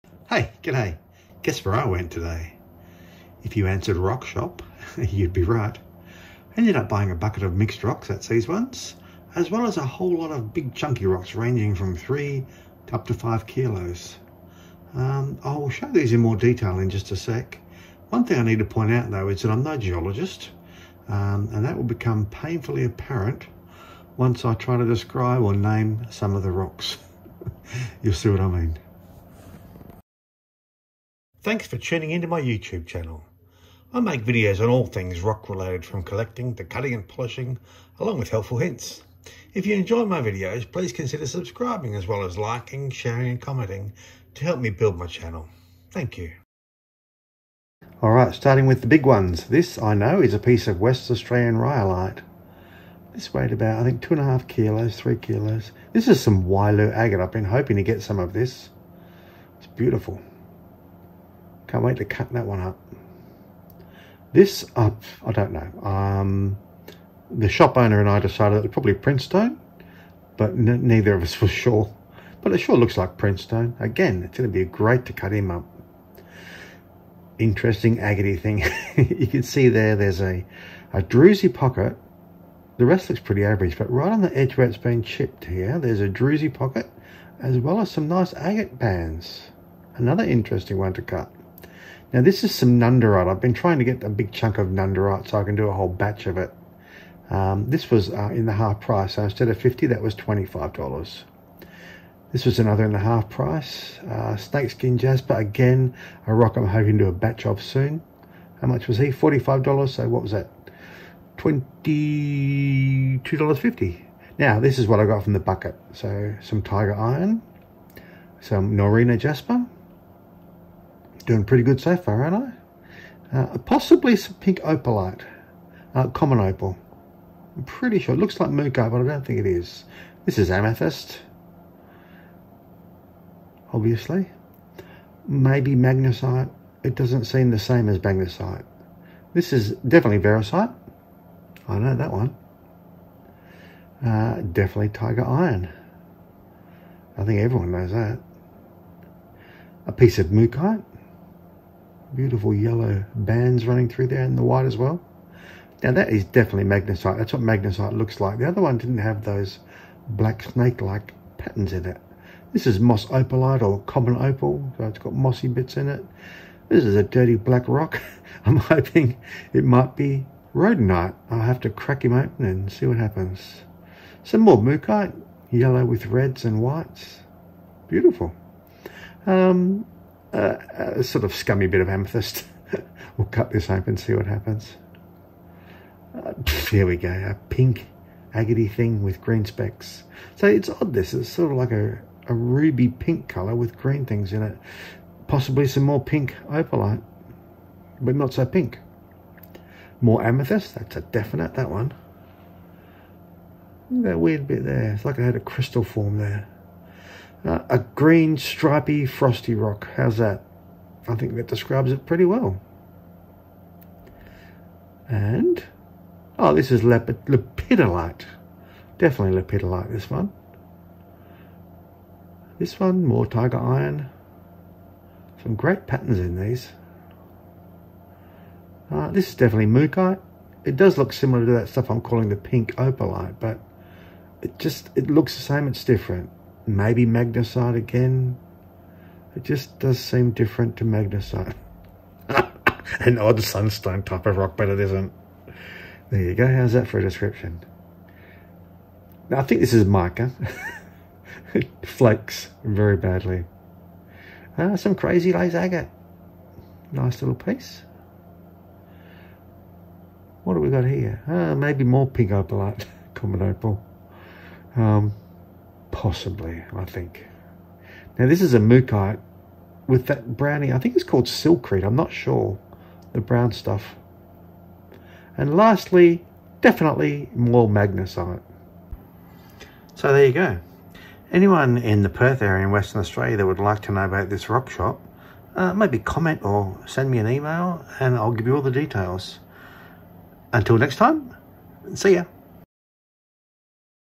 Another Visit to the Rock Shop
Wowee . . . some magnificent pieces from the half price table at the rock shop today including Nundoorite, West Australian Rhyolite, Wyloo Agate, Printstone, Starry Jasper, Noreena Jasper, Amethyst, Magnesite, Crazy Lace Agate, Mookaite and many others.